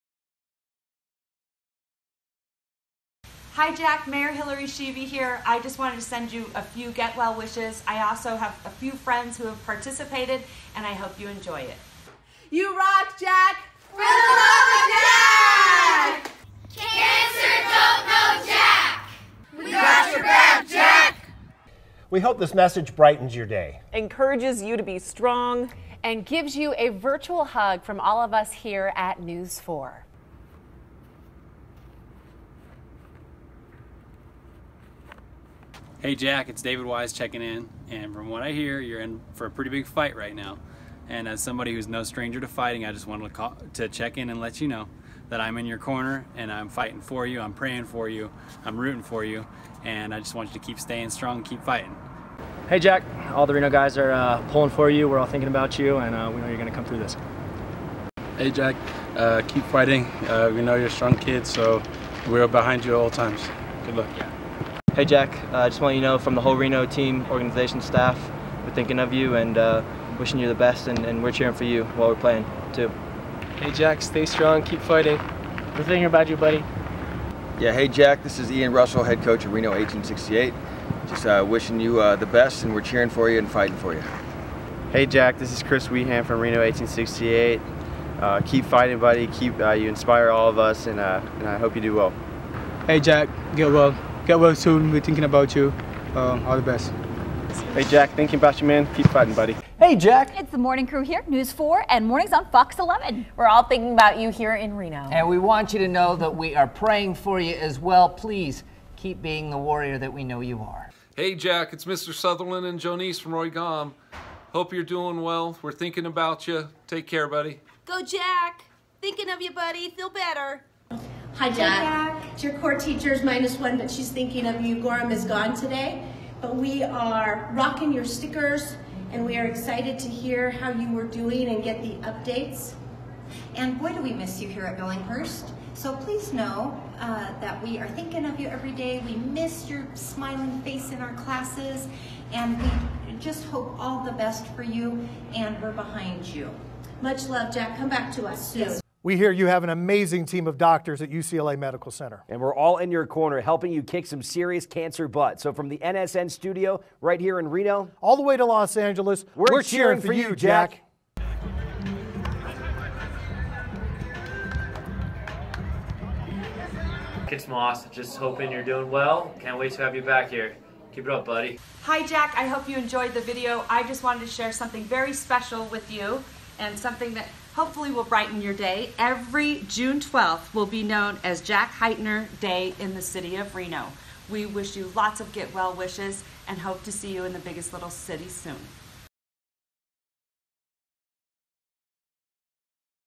Cancer Don't Know Jack
Mayor Hillary Schieve and friends give all their love and support to Jack as he keeps fighting. He will win!